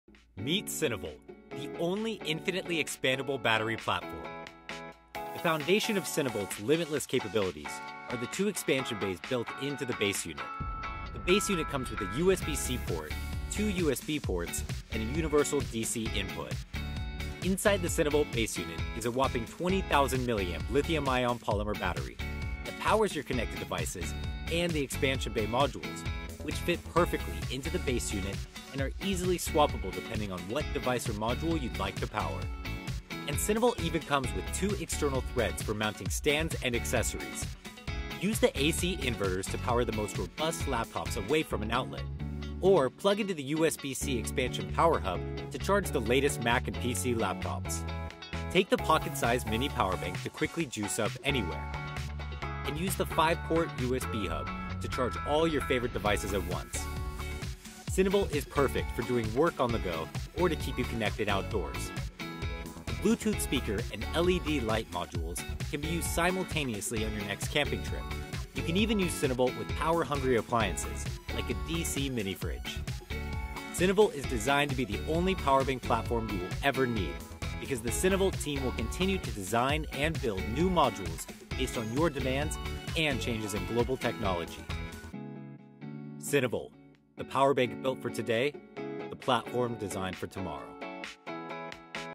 SYNAVOLT - The Expandable Modular 20K mAh Power bank
Get all the ports you need to power your gadgets and ditch the rest! SYNAVOLT is the next generation of power bank.